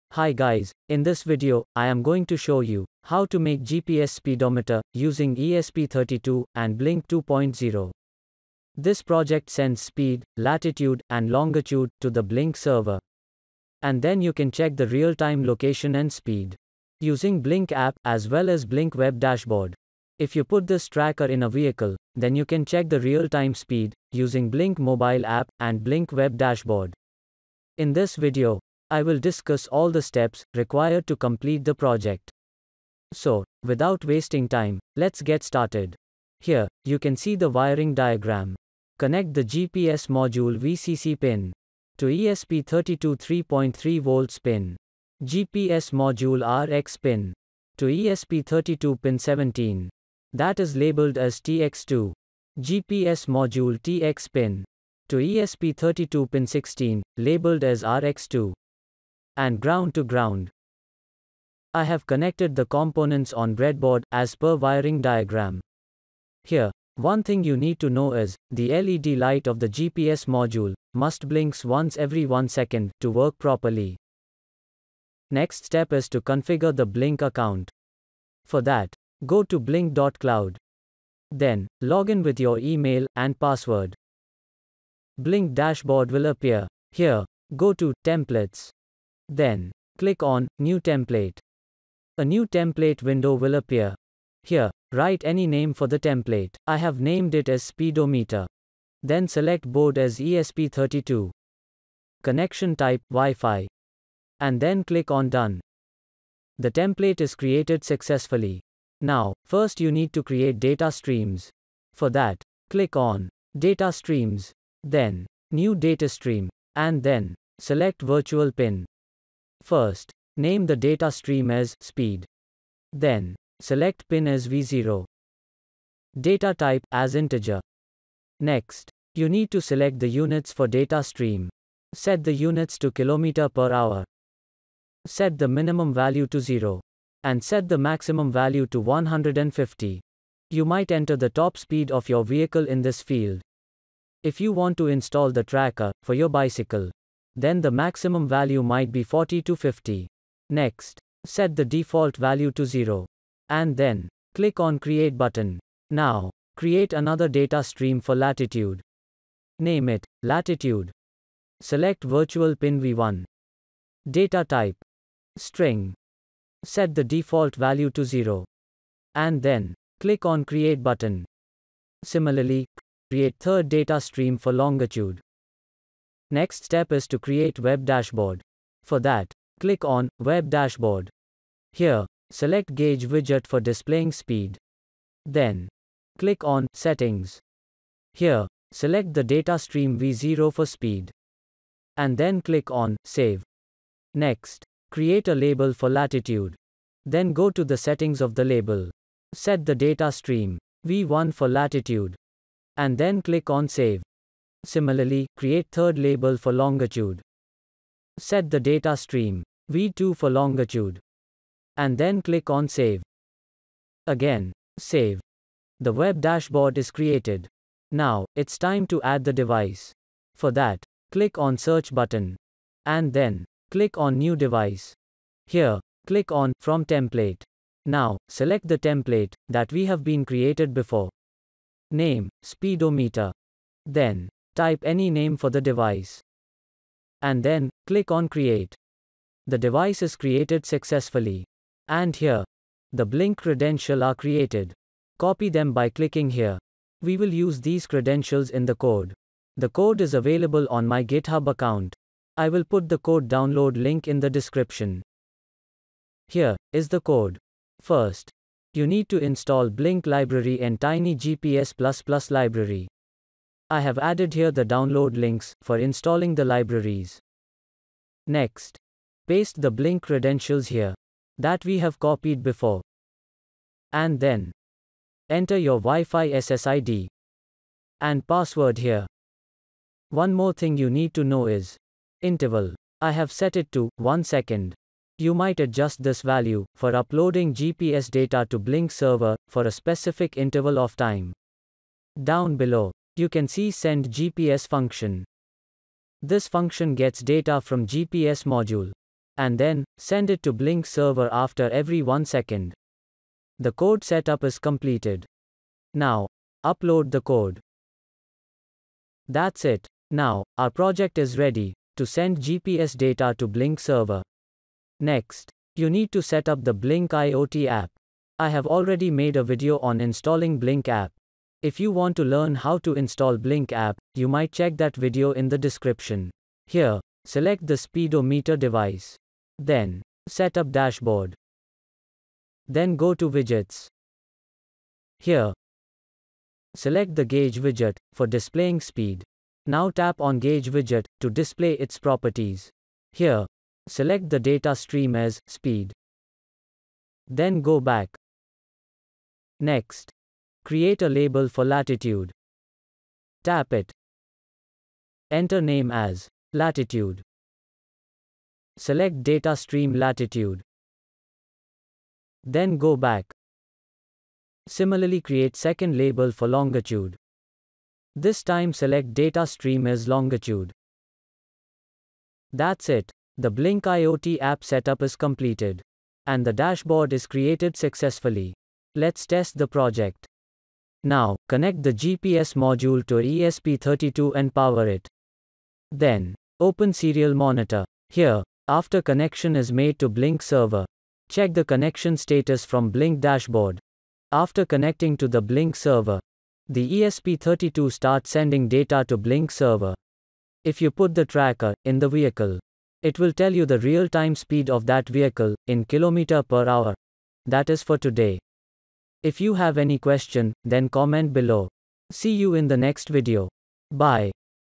ESP32 GPS Speedometer using Blynk 2.0 | Blynk GPS Tracker
I am going to show you, how to make ESP32 GPS Speedometer, using blynk 2.0. This project sends speed, latitude, and longitude, to the blynk server. and then you can check the real time location and speed using Blynk App, as well as Blynk web dashboard. if you put this Blynk GPS Tracker in a vehicle, then you can check the real time speed, using blynk mobile app, and blynk web dashboard. in this video,

I will discus all the steps, required to complete the project. So, without wasting time, let's get started.

Parts:
1. ESP32
2. NEO-6M GPS Module
3. Breadboard
4. Jumper Wires